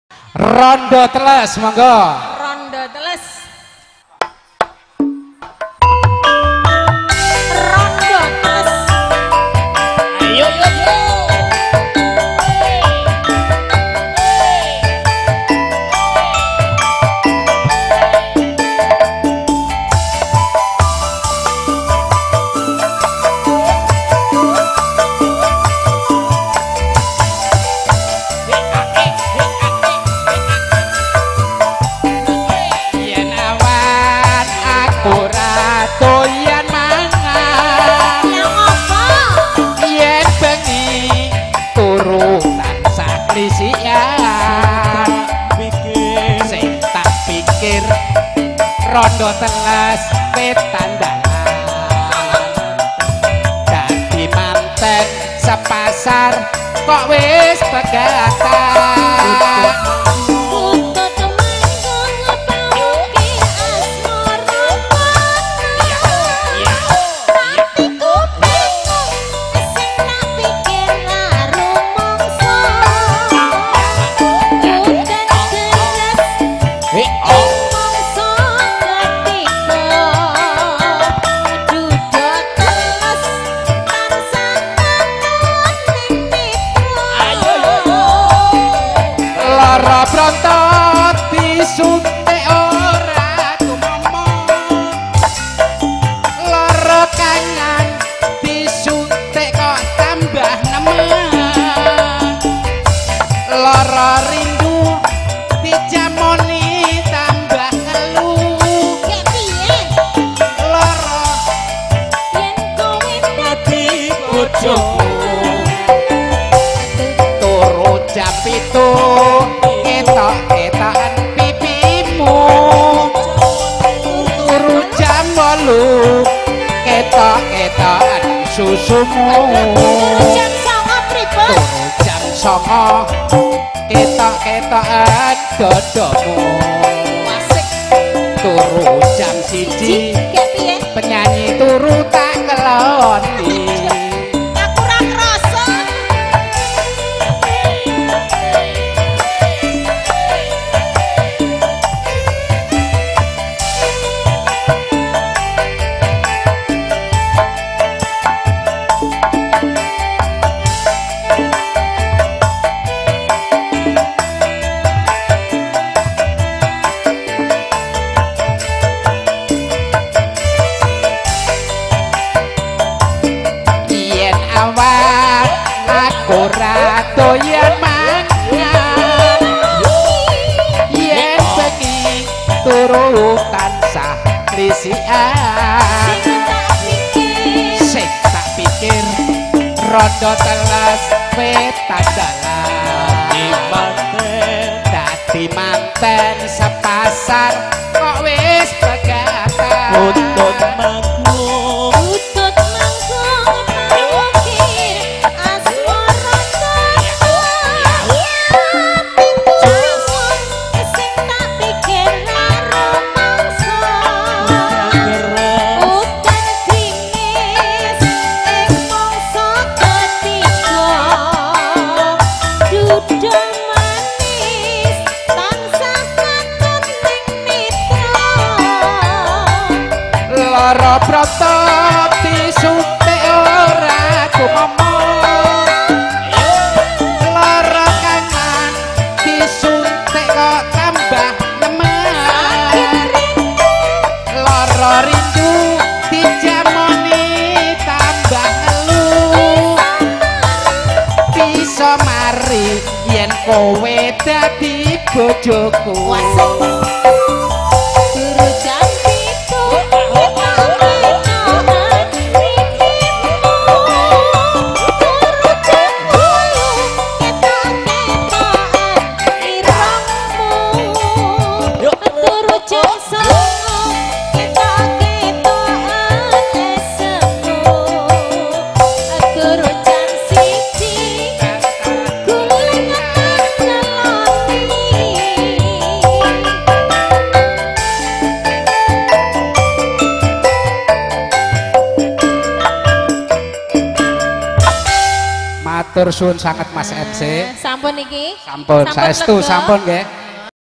Rondo Teles Di Resepsi Pernikahan
Live Show Sampurna Musik dalam rangka memeriahkan resepsi pernikahan dr. Siti Nandiroh & dr. Alfriyandi.
Sitiung. III Blok. C Nag. Kurnia Selatan Kec. Sungai Rumbai Kab. Dharmasraya Sumatera Barat.
Ok, guys. selamat menikmati moga terhibur.
Salam sejahtera selalu di manapun berada dan kita selalu dalam lindungan Allah SWT.